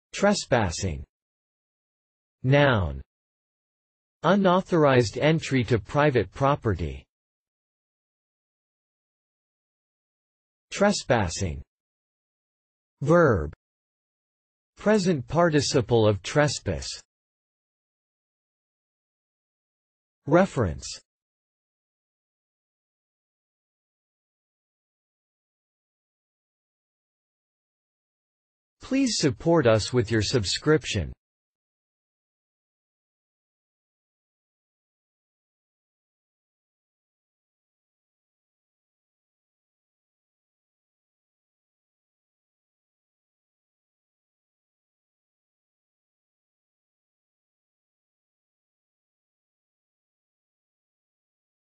Trespassing | Meaning of trespassing 📖
See here, the meanings of the word trespassing, as video and text.

trespassing (noun)
    Unauthorized entry to private property.

trespassing (verb)
    Present participle of trespass.

Reference:
    trespassing